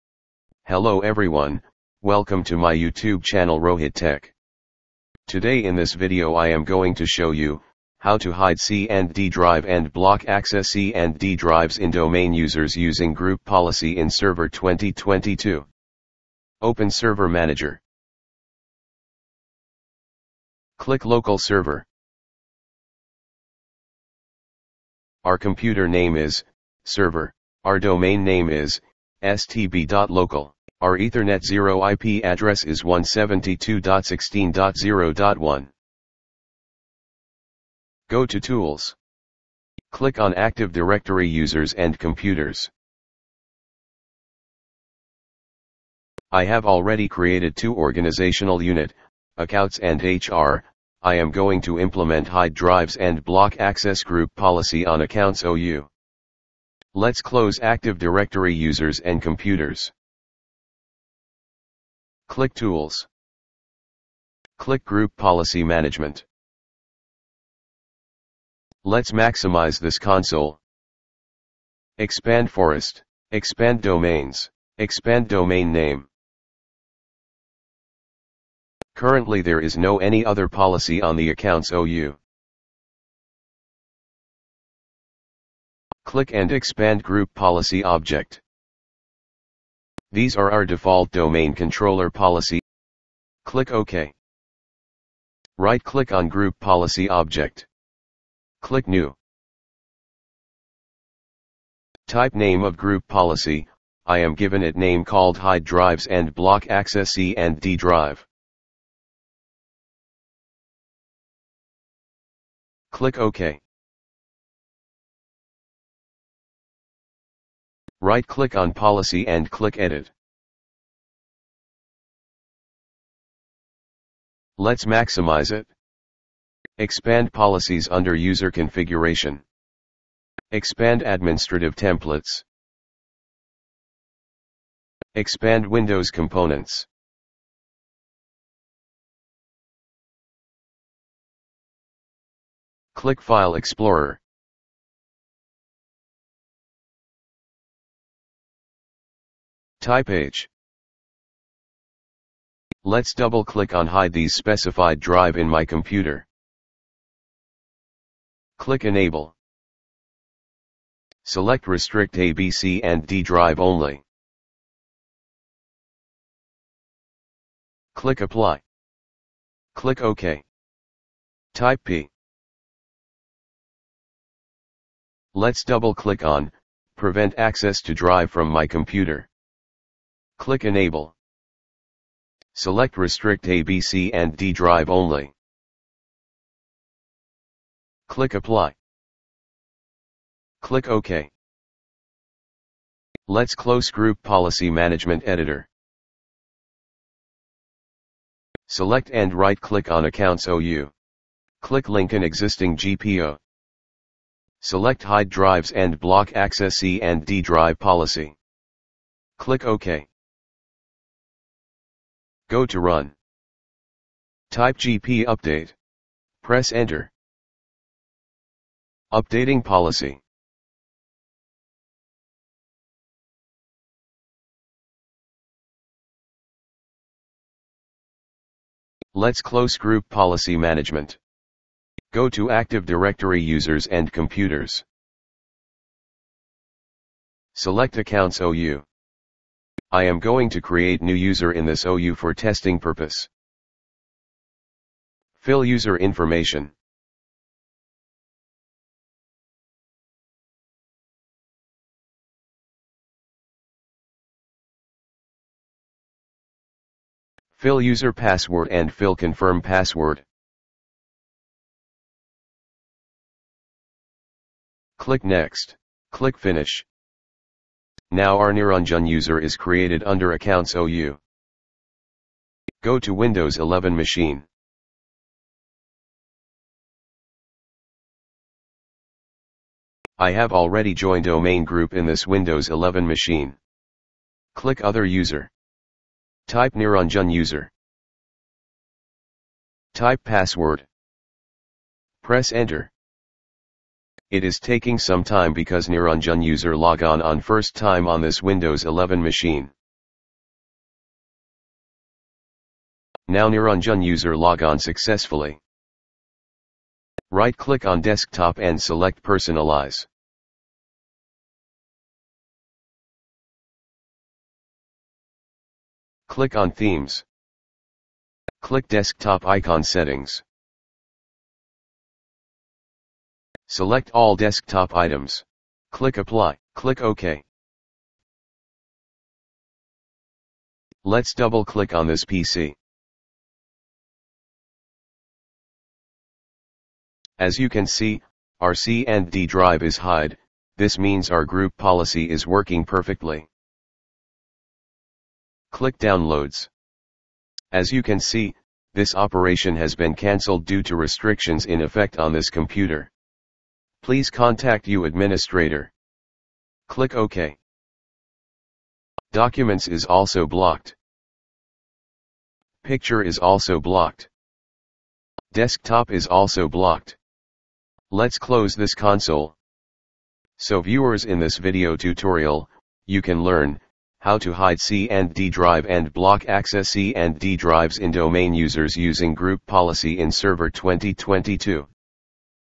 How to Hide & Block Access C & D drives Using Group Policy in Domain Users on Server 2022 !!
This Policy also tested on Windows server 2019 & 2016
How to Block Desktop, user cannot save data on desktop using group policy